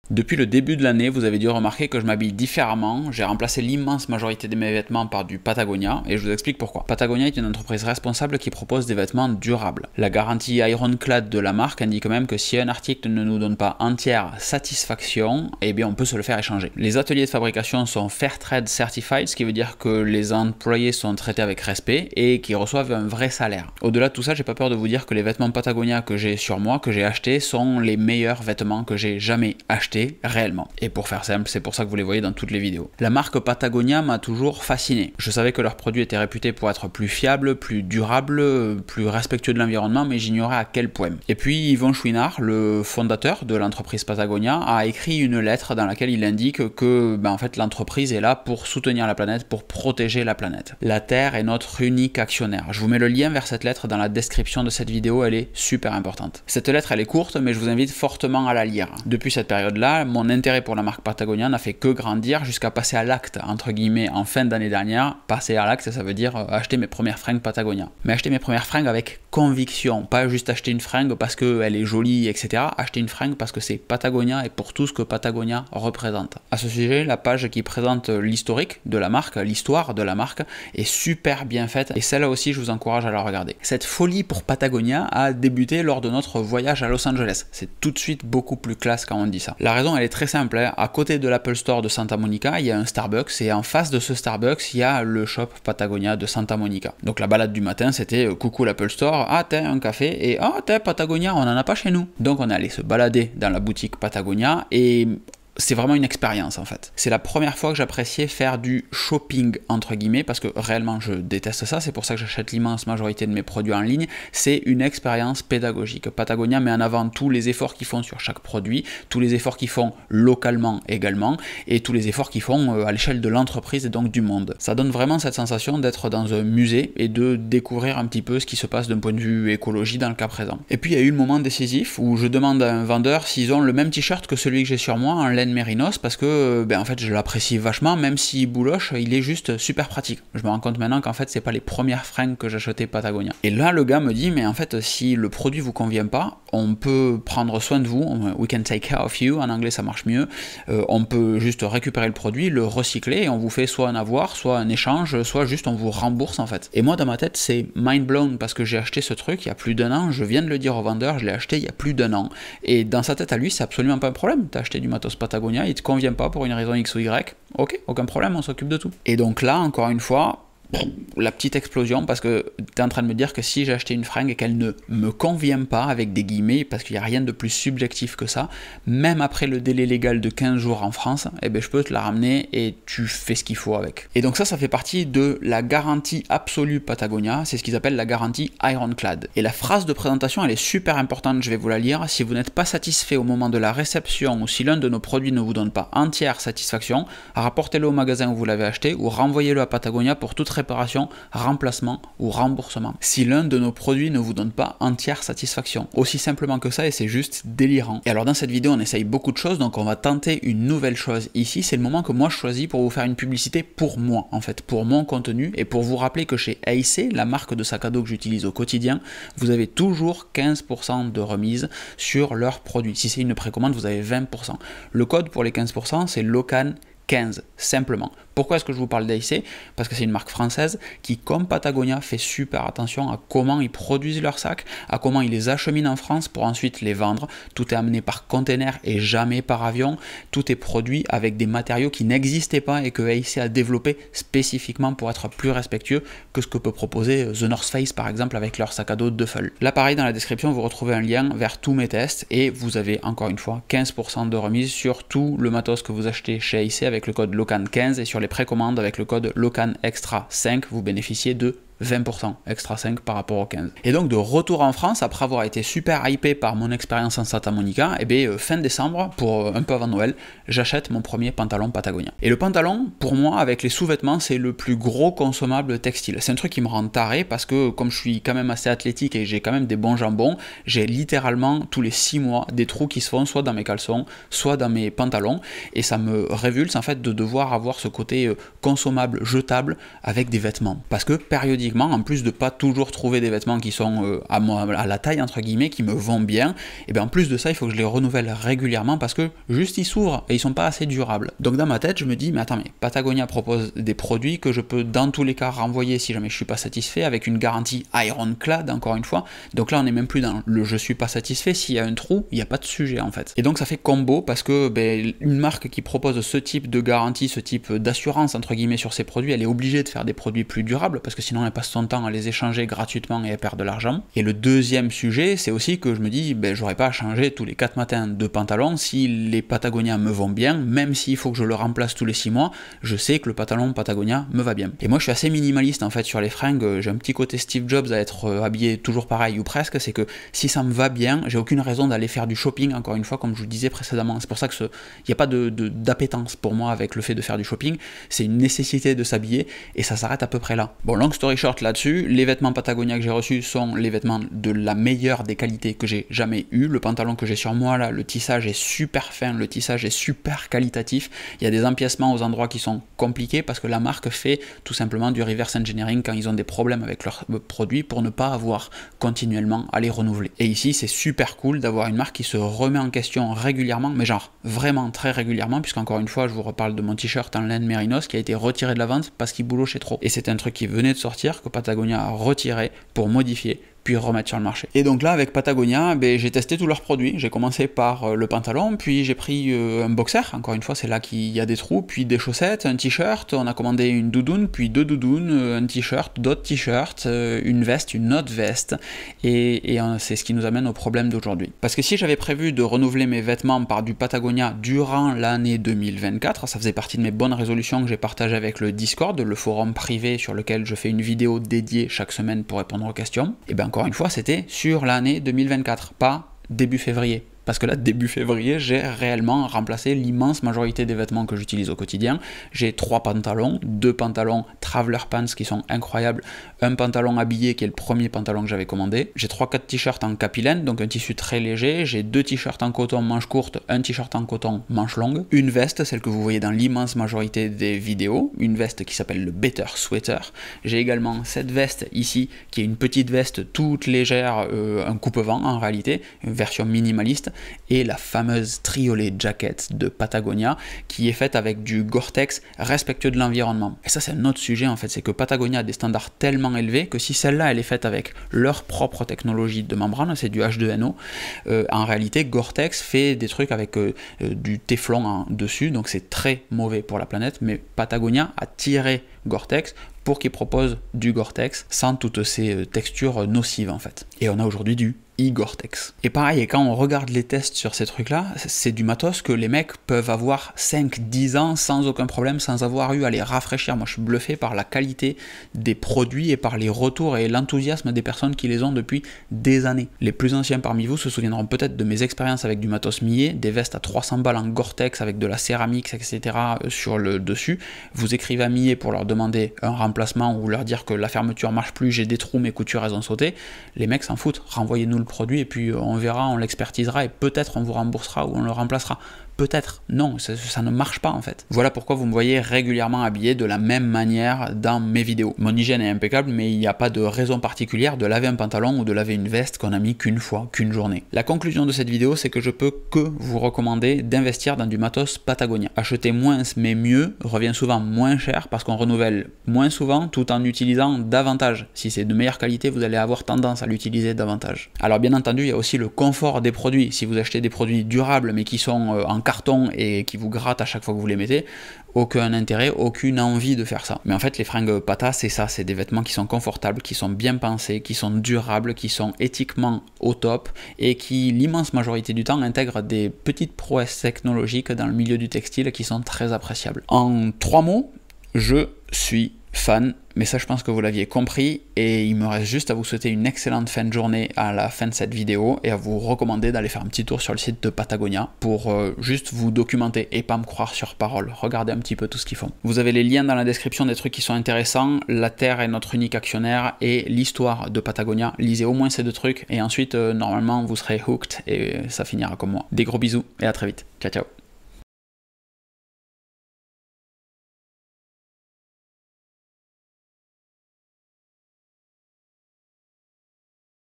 Je remplace TOUTES mes fringues par du Patagonia
Depuis le début de l’année, vous avez dû remarquer que je m’habille différemment. J’ai remplacé la majorité de mes vêtements par du Patagonia et je vous explique pourquoi.

NOTES ⤵️

BONS PLANS ⤵️